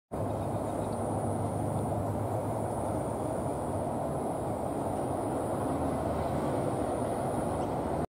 Egret looking for food among aquatic plants
Egret bird looking for food among aquatic plants.